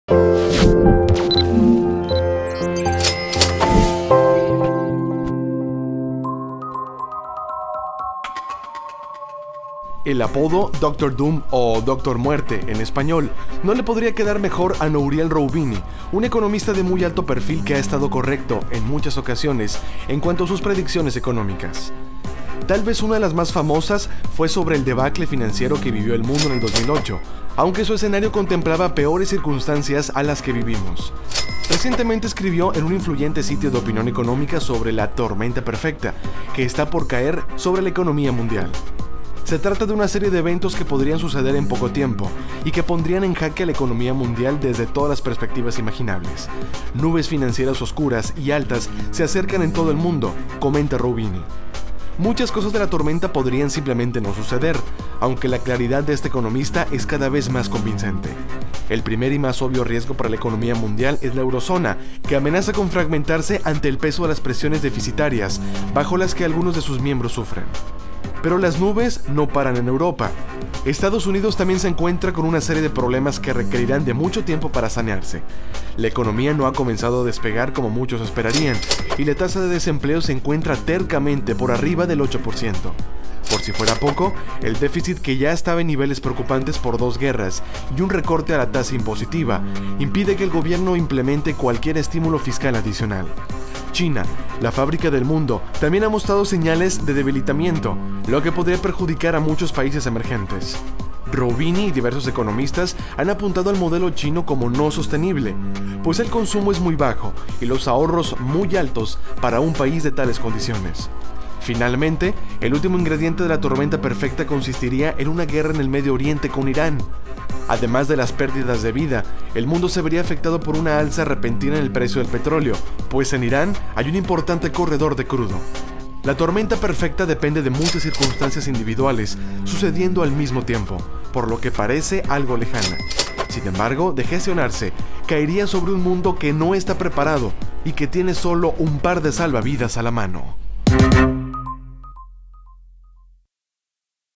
Reporte Indigo (Edición 82): La tormenta que viene
Una serie de eventos, según un influyente economista, podría desencadenar problemas económicos sin precedentes. La nota completa